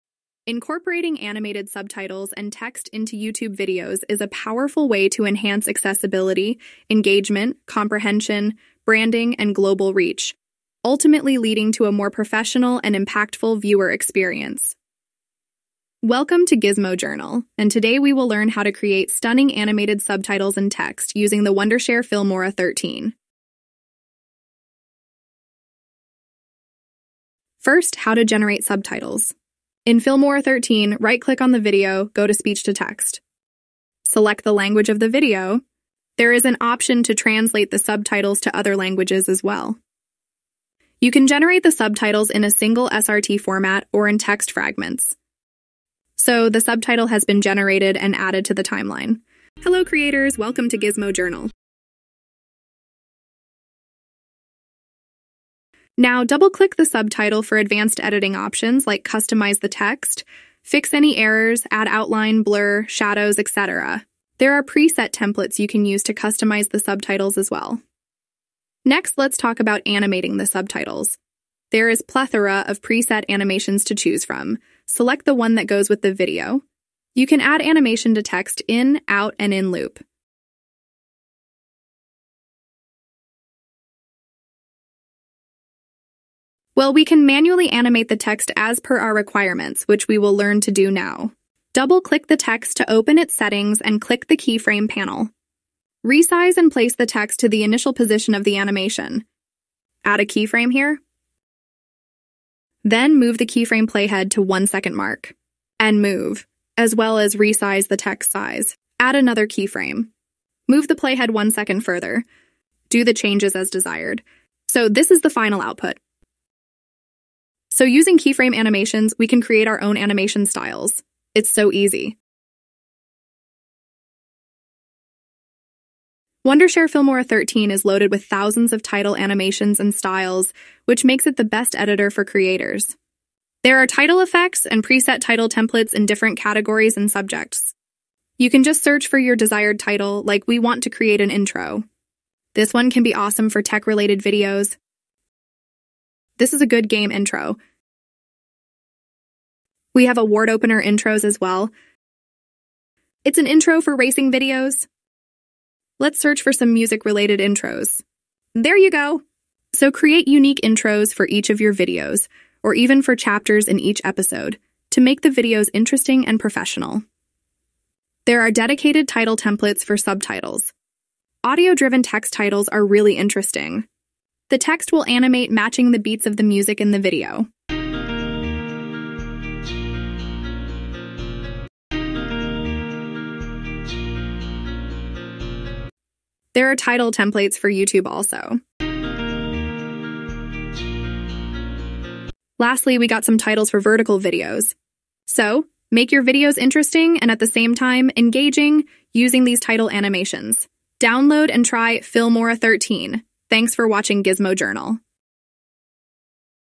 Complete Guide to Creating Stunning Animated Subtitles & Text using Filmora 13
Incorporating animated subtitles and text into YouTube videos is a powerful way to enhance accessibility, engagement, comprehension, branding, and global reach, ultimately leading to a more professional and impactful viewer experience.
a. Generating subtitles using AI Speech to Text
b. Animating subtitles using built-in animation templates: In, out and loop. Duration control.
c. Animating subtitles and text using Keyframes.
d. Title animation using pre-loaded effects and templates. Finding the perfect title for Intros, branding, logo, chapters, message, festival, social media, end credits, music, end screen.